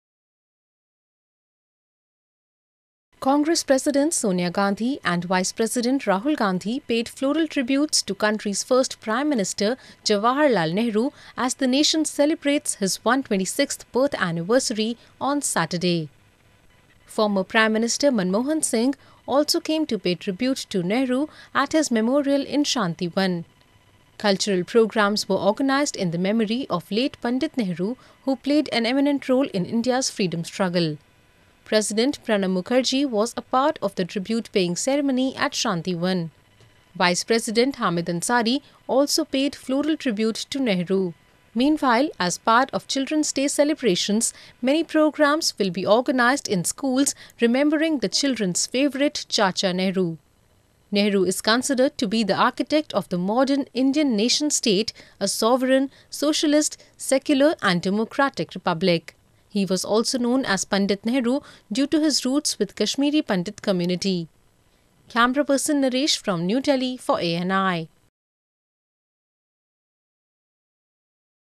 Leaders pay floral tributes on Nehru’s birth anniversary
New Delhi, Nov 14 (ANI): Congress President Sonia Gandhi and Vice President Rahul Gandhi paid floral tributes to country’s first Prime Minister Jawaharlal Nehru as the nation celebrates his 126th birth anniversary on Saturday. Former PM Manmohan Singh also came to pay tribute to Nehru at his memorial in Shantivan. Cultural programs were organised in the memory of late Pandit Nehru who played an eminent role in India’s freedom struggle. President Pranab Mukherjee was a part of the tribute paying ceremony at Shantivan. Vice President Hamid Ansari also paid floral tribute to Nehru. Meanwhile, as part of children's day celebrations, many programmes will be organised in schools remembering the children’s favourite ‘Chacha Nehru’. Nehru is considered to be the architect of the modern Indian nation-state: a sovereign, socialist, secular, and democratic republic. He was also known as Pandit Nehru due to his roots with Kashmiri Pandit community.